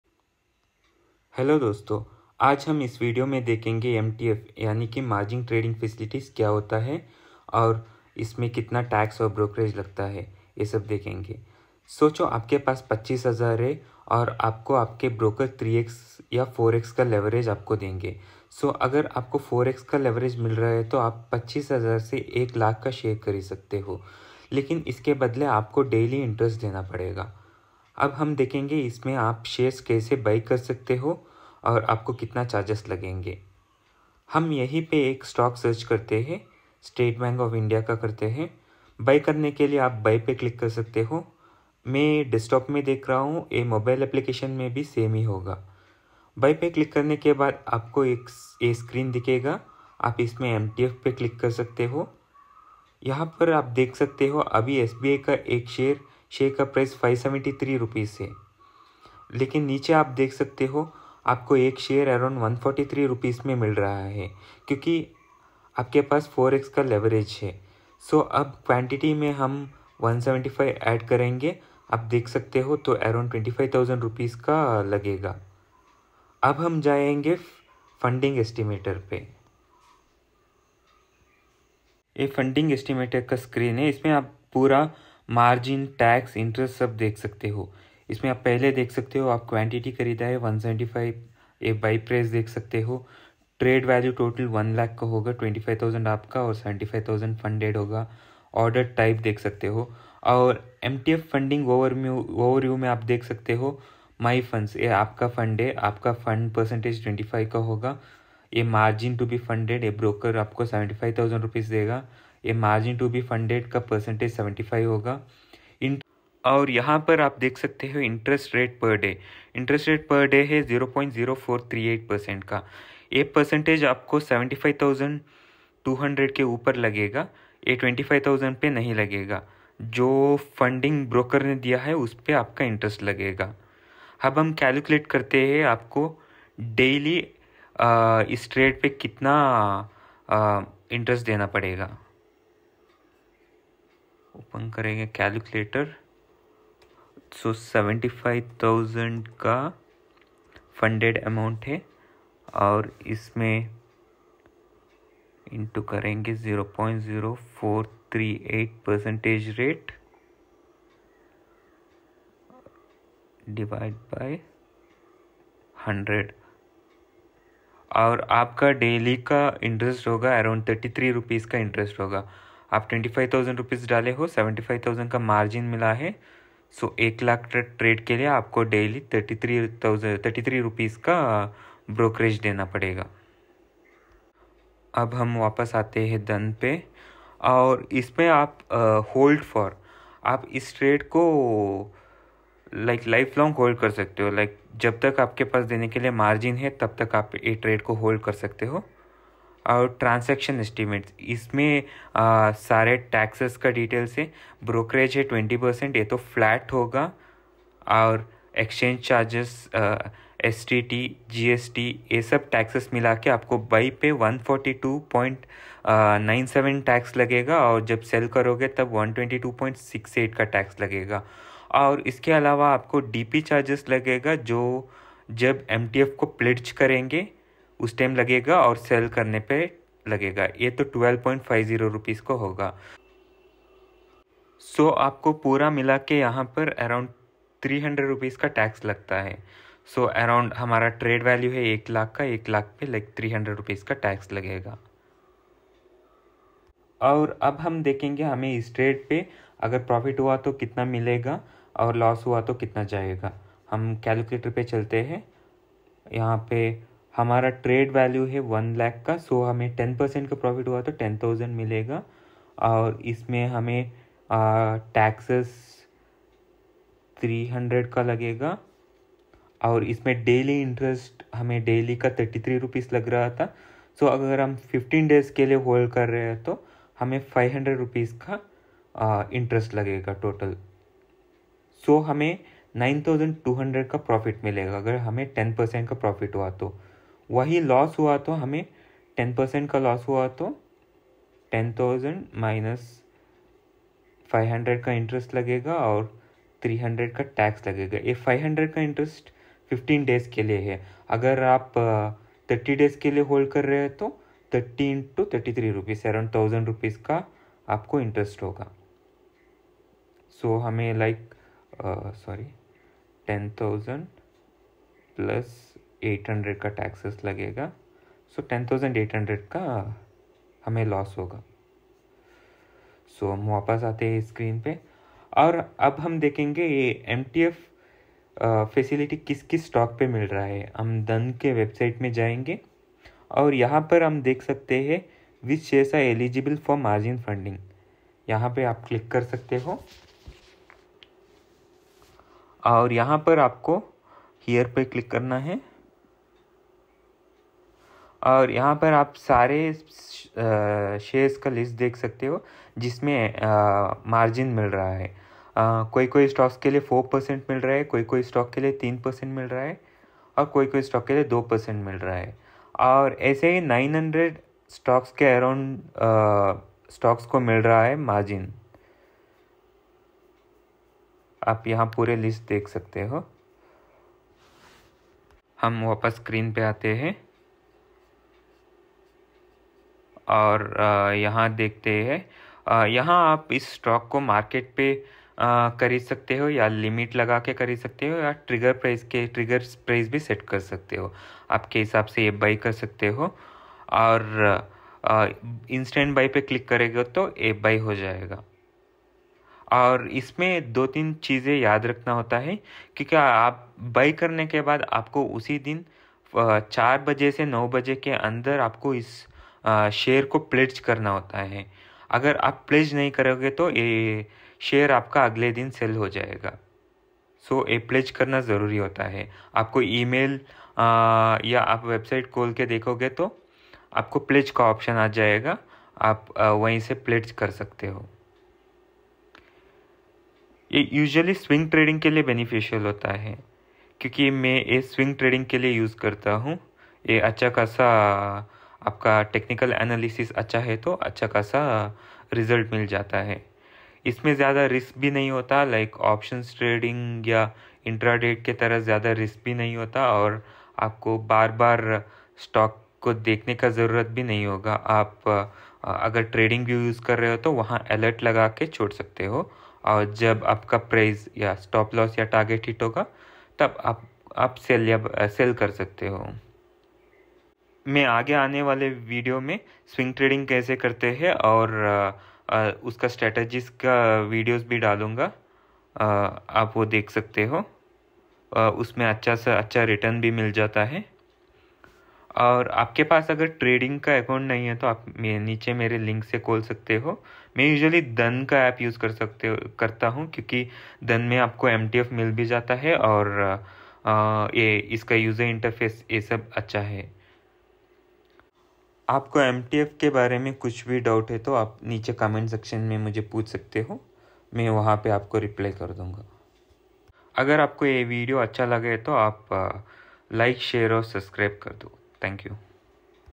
Taxes and Brokerage on Margin trading facility | MTF in Dhan |complete details
Margin trading facility. MTF in dhan complete details. leverage trading on dhan. interest on mtf